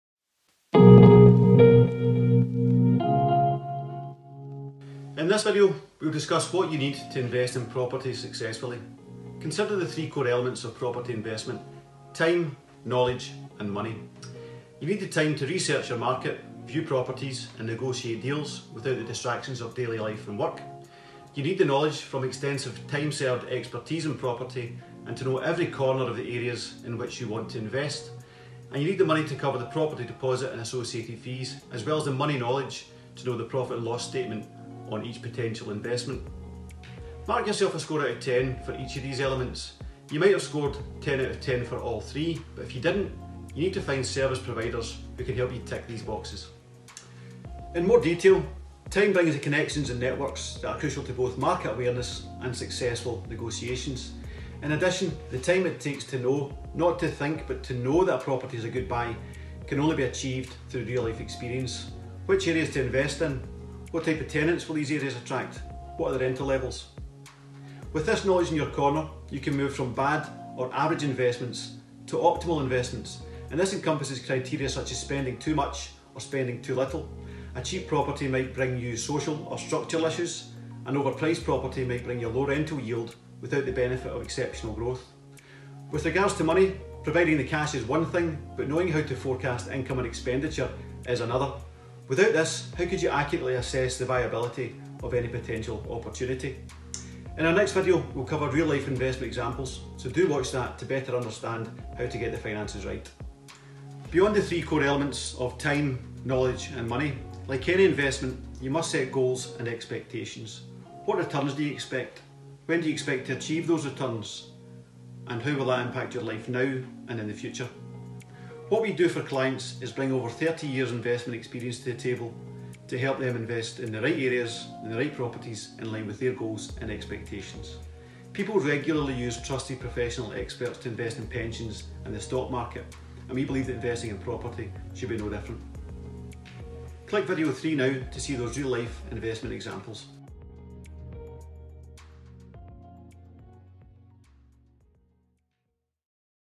How to Invest in Property Successfully
There are three key resources required to invest in property successfully.